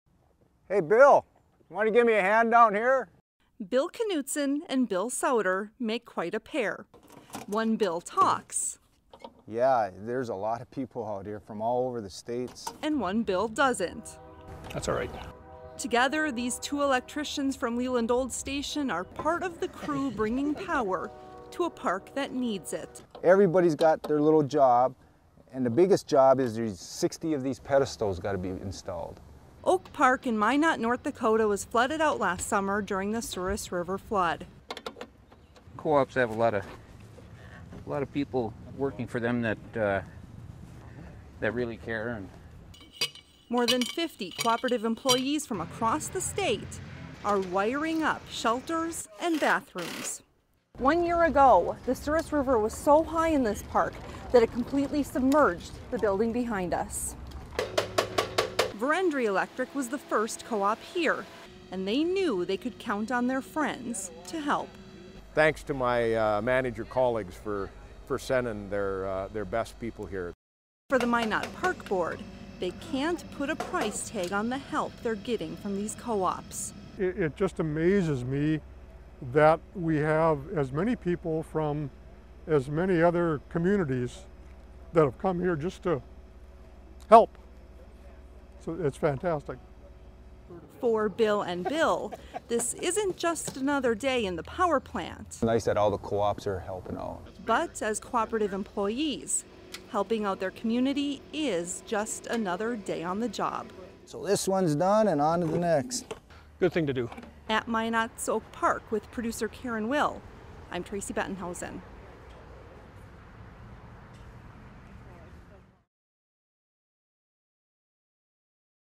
Electric co-op employees power up Oak Park in Minot, ND
(Bill Knudson): "Hey Bill, you wanna give me a hand down here?"

Bill Knudson and Bill Sauter make quite a pair. One Bill talks...

(Bill Knudson): "Yah, there's a lot of people out here from all over the state."

...And one Bill doesn't.

(Bill Sauter): That's okay.

Together, these two electricians from Leland Olds Station are part of the crew bringing power to a park that needs it.

(Bill Knudson): "Everybody's got their little job and the biggest job is there's 60 of these pedestals that gotta be installed."

Oak Park in Minot, ND, was flooded out last summer during the Souris River flood.

(Bill Knudson): "It's so stressful for these people that have been flooded out."

(Dale Howard): "Co-ops have a lot of people working for them that really care."

More than 50 cooperative employees from across the state are wiring up shelters and bathrooms.

(Tracie Bettenhausen): "One year ago, the Souris River was so high in this park that it completely submerged the building behind us."

Verendrye Electric was the first co-op here... and they knew they could count on their friends to help.

(Bruce Carlson): "Thanks to my manager colleagues for sending their best people here."

For the Minot Park Board, they can't put a price tag on the help they're getting from the co-ops.

(Ron Bieri): "It just amazes me that we have as many people from as many other communities that have come here just to help. It's fantastic."

For Bill and Bill, this isn't just another day in the power plant.

(Bill Knudson): "It's nice that all the co-ops are helping out."

But as cooperative employees, helping out their community is just another day on the job.

(Bill Sauter): It's a good thing.

(Bill Knudson): "This one's done and on to the next."